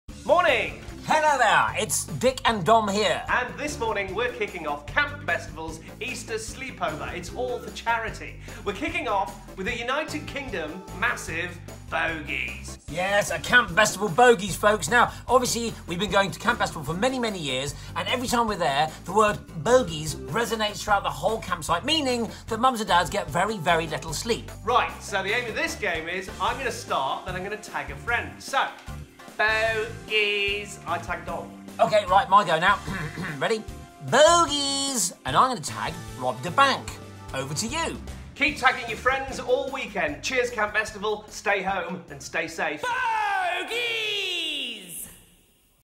Dick & Dom's Campsite Bogies!
Uh oh...watch out Camp Bestival, it’s the UK’s flag flyers of chaos Dick and Dom!! The CB legends will be kicking off our Sleepover with a nationwide game of BOGIES, so get ready to tag your family and friends and see who can shout the loudest…